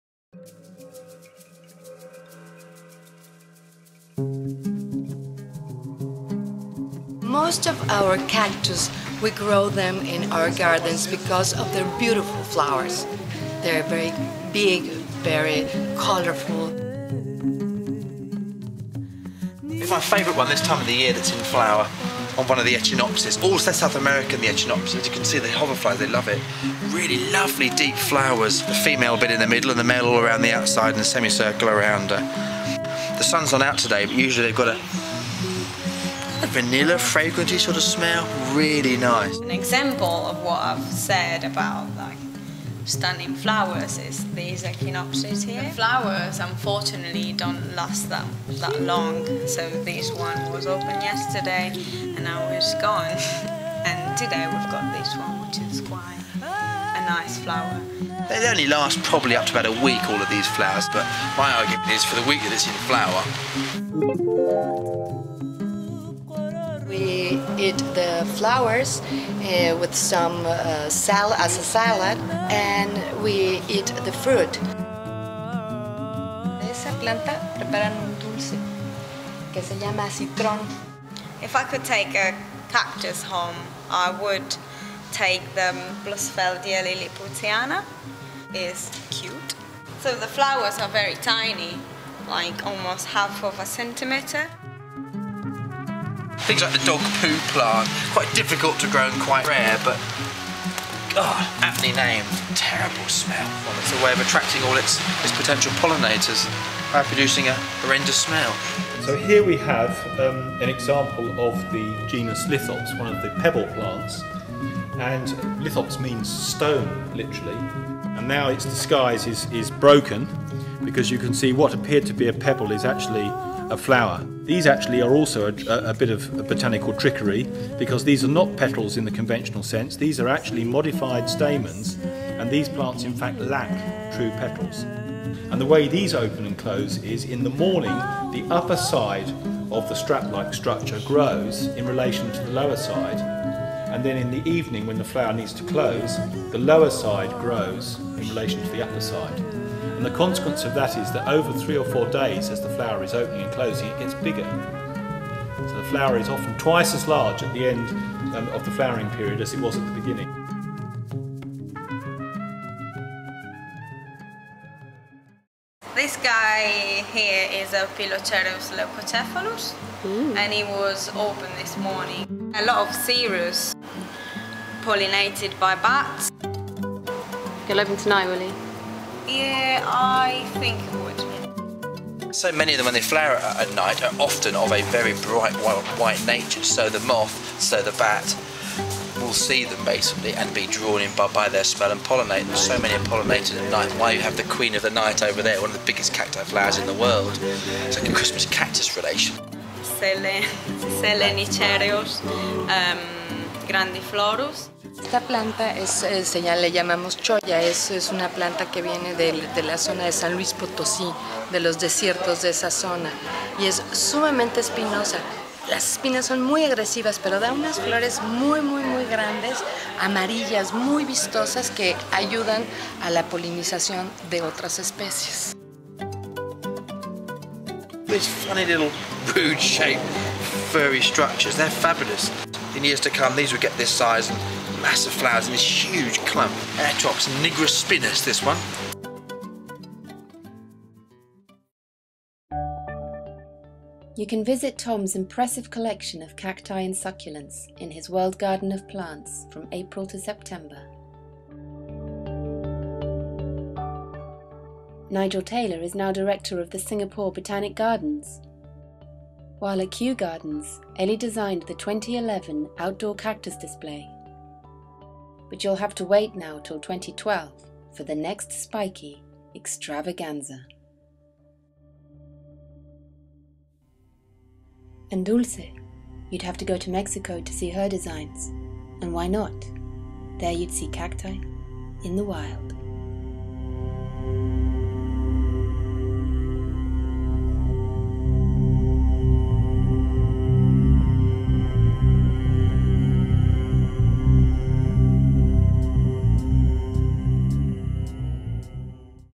Cactus Flowers
What's special about cactus flowers? I ask the experts Dr. Nigel Taylor, Tom Hart Dyke, Elisa Biondi and Dulce Belmar, in my personal quest to understand the peculiar world of cacti. This clip is part of a series on cacti plants. Please note that the Lithops genus is not part of the Cactaceae family, but being succulents (like the well-known aloes and agaves), they are considered distant cousins, if you like..